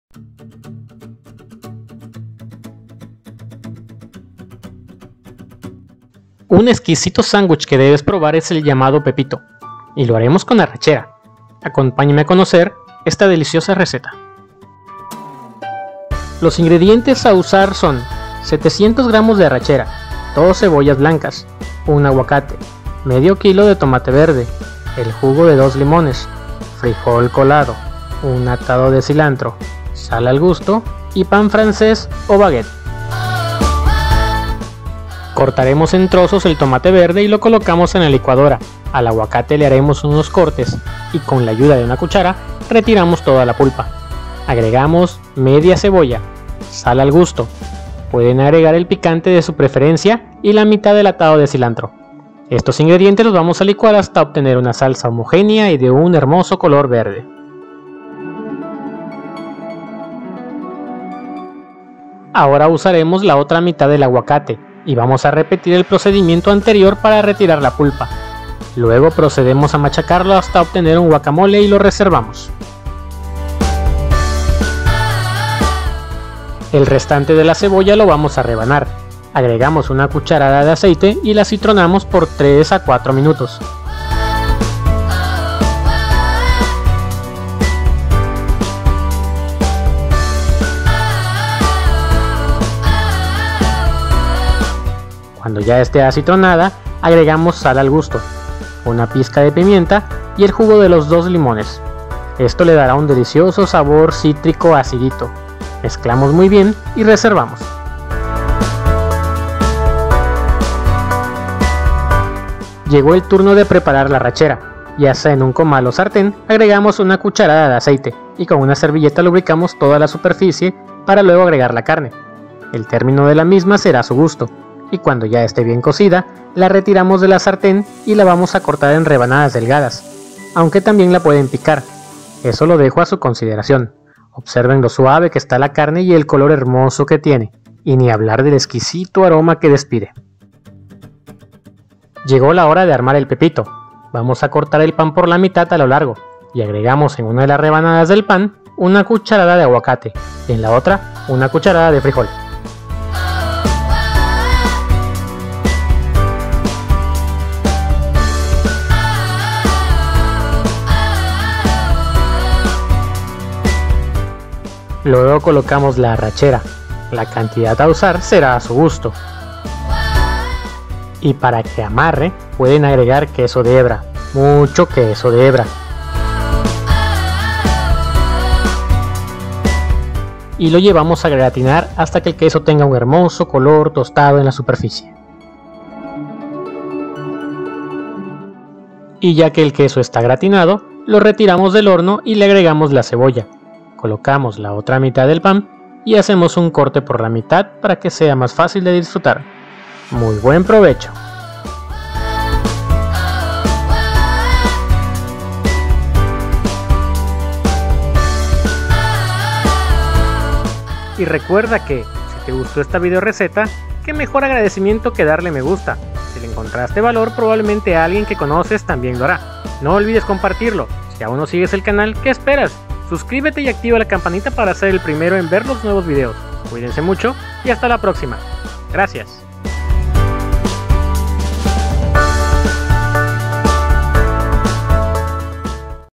como hacer PEPITO DE ARRACHERA | SANDWICH
ingredientes
arrachera, o la carne de su preferencia
500 gramos de tomate verde
1 pieza de aguacate
2 cebollas
1 atado de cilantro
sal al gusto
el jugo de uno o dos limones
pan frances o Baguette
frijol colado